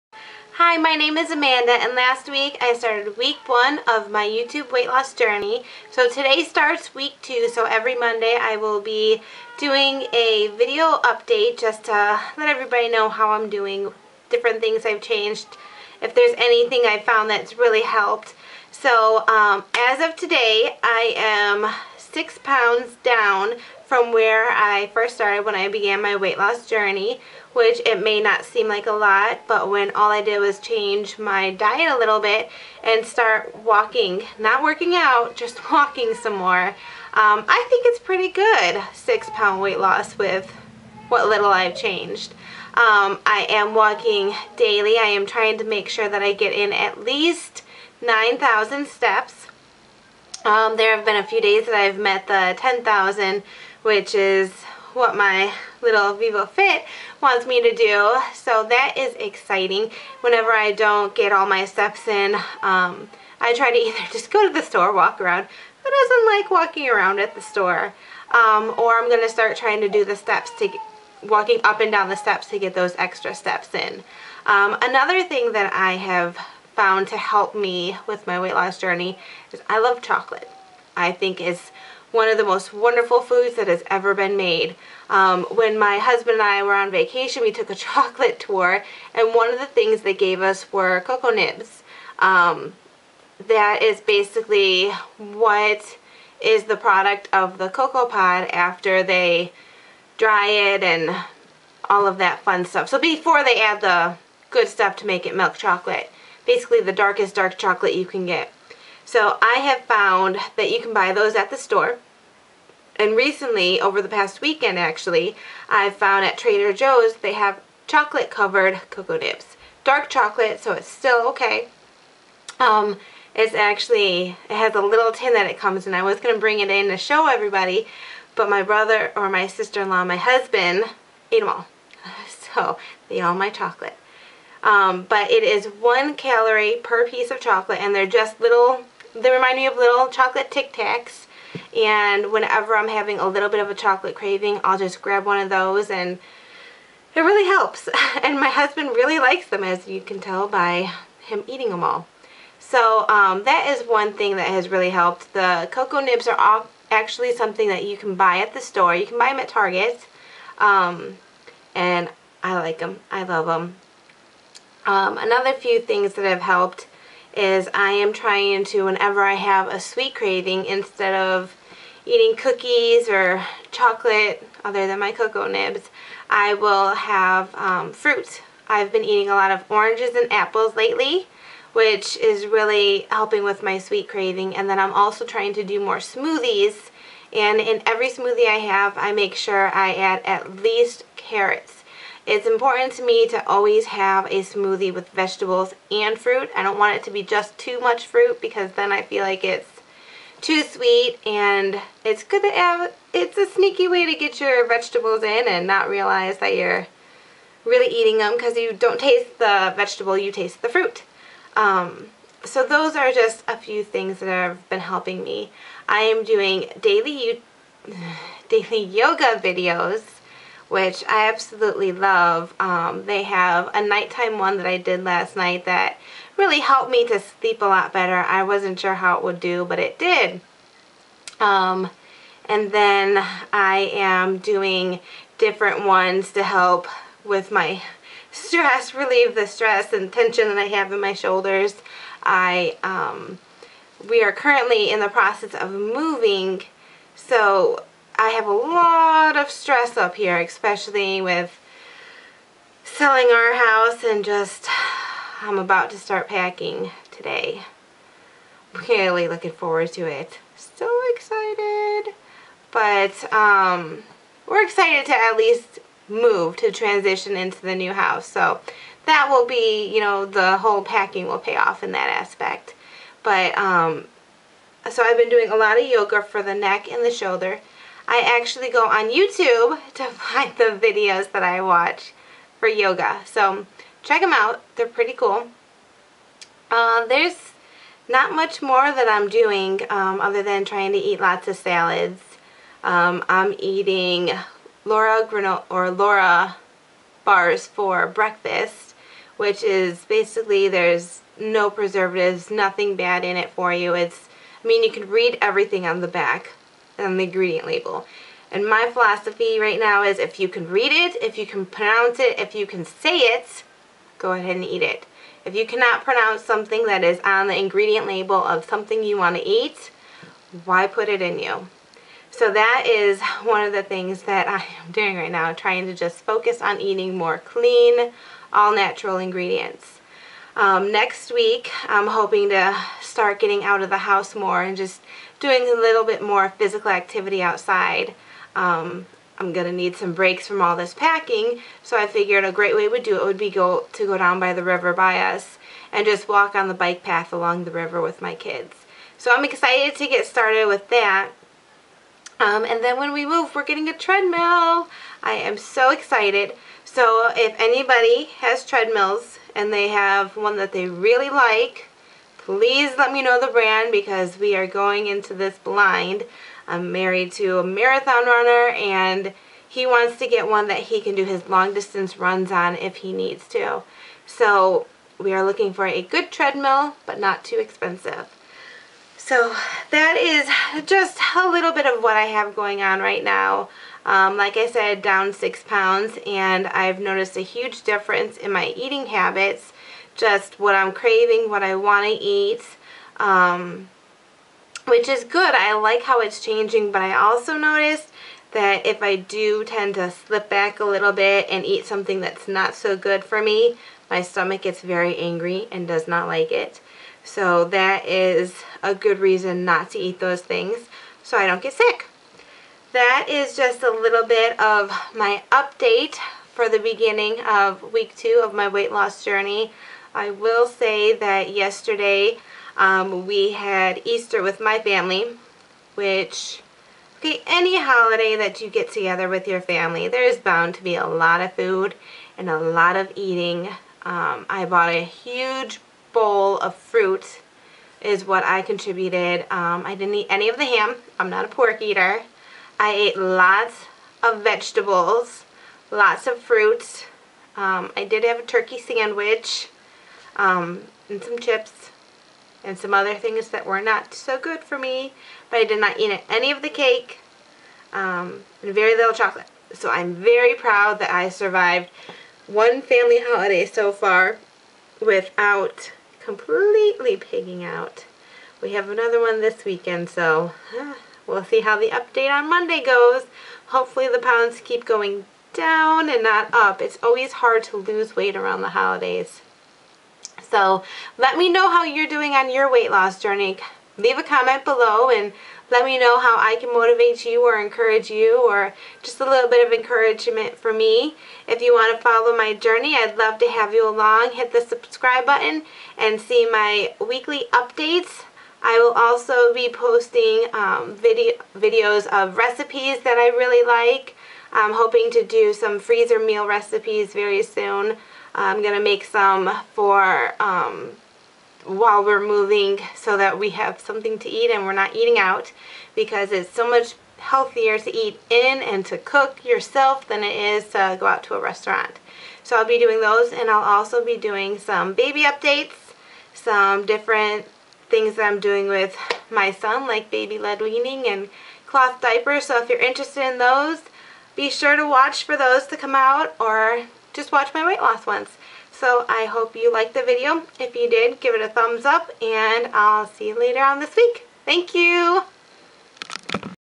Weight loss week 2
Here it is week 2 of my weight loss journey.  Just an update on how week 1 went.

Follow me on MyFitnessPal: amanda915
                     Pintrest: amanda915